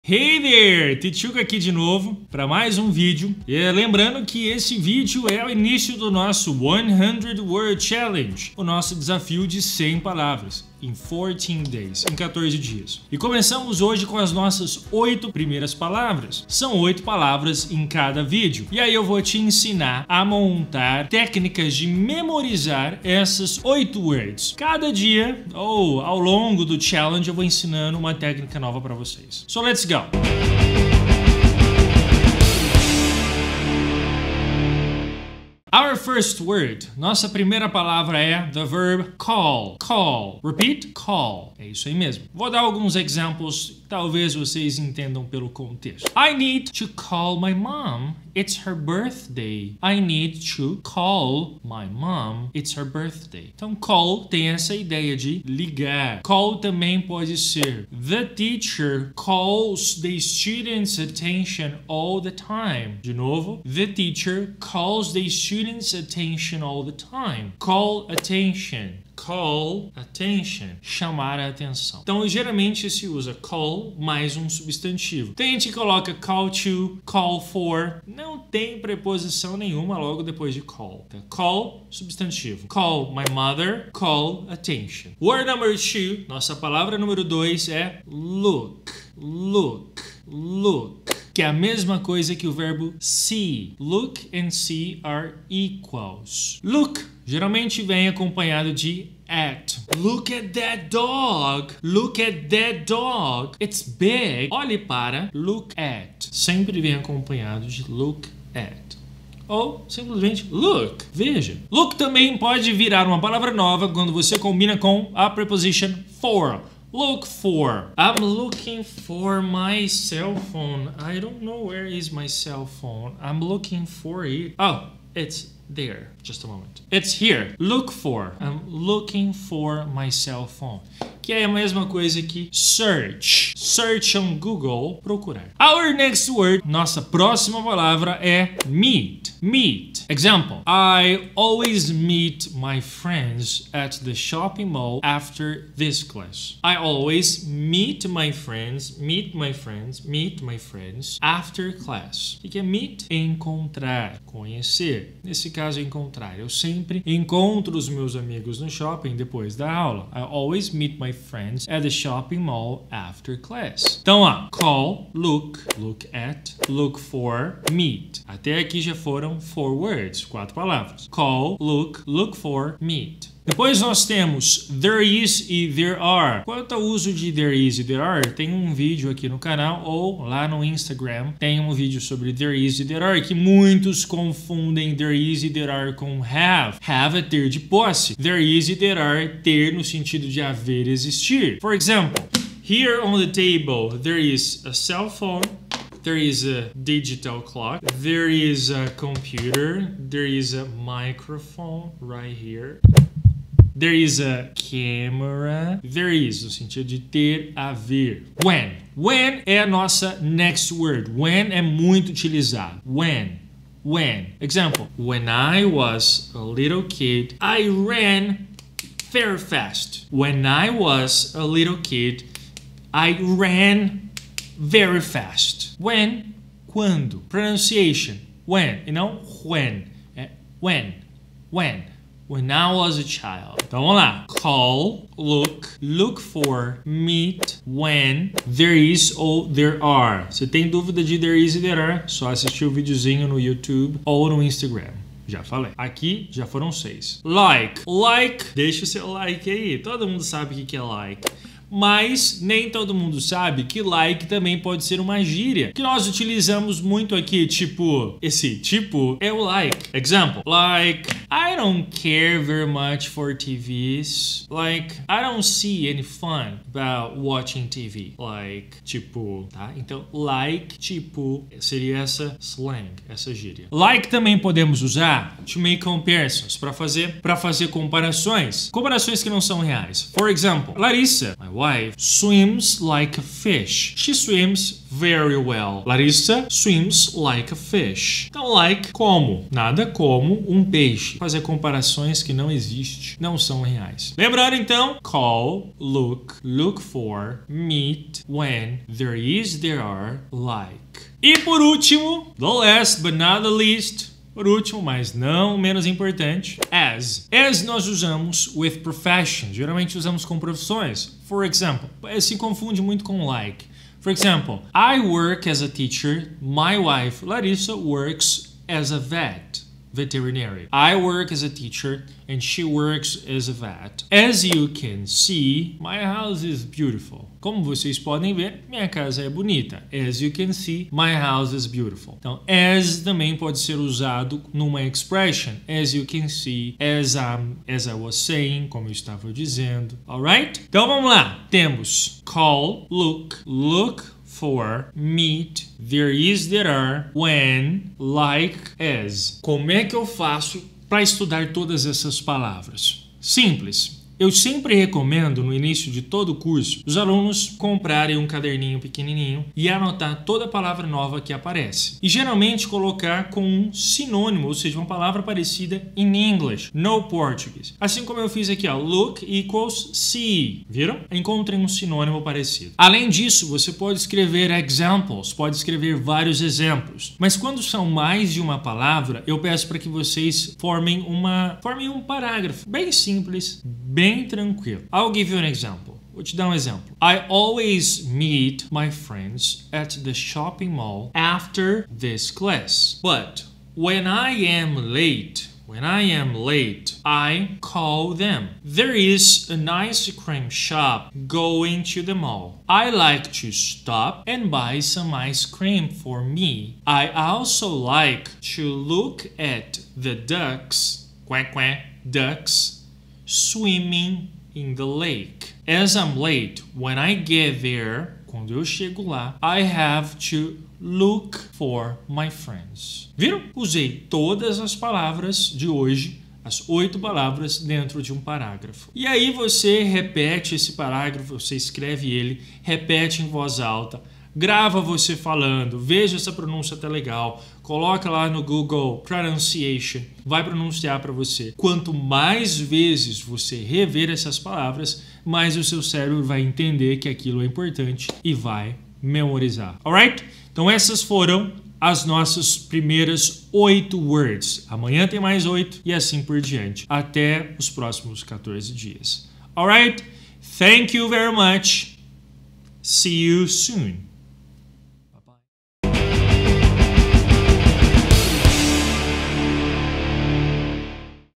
100-word Challenge
Olá. tudo bom?

Segue o primeiro da série de videos do 100-word challenge.
Nesse desafio vamos aprender 100 palavras, sendo elas phrasal verbs, expressions, prepositions, etc.

Serão 8 palavras por dia.

Curta, Compartilhe e aprenda.